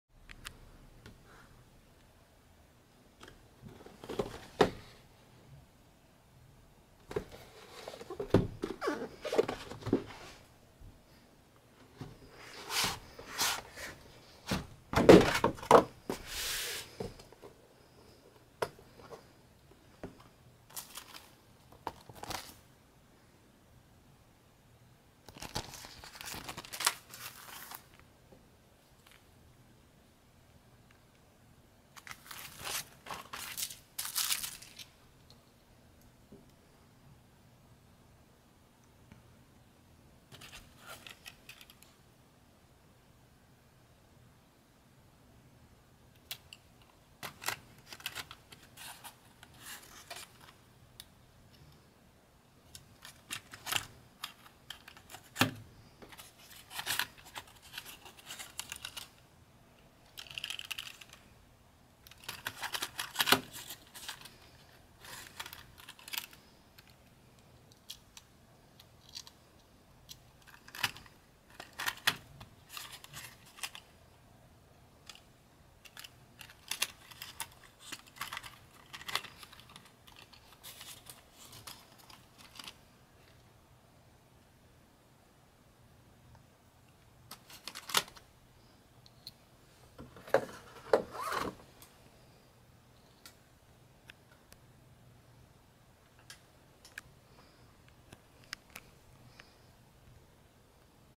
Nゲージ カトー阪急6300系6354F京とれいん 開封
ホビーセンターカトーから少数だけ発売された10-941 阪急6300系6354F京とれいんです。
予約していなかったので、買えるかどうかわかりませんでしたがポポンデッタで最後の1セットが何とか買えました。
製造元はKATOみたいです。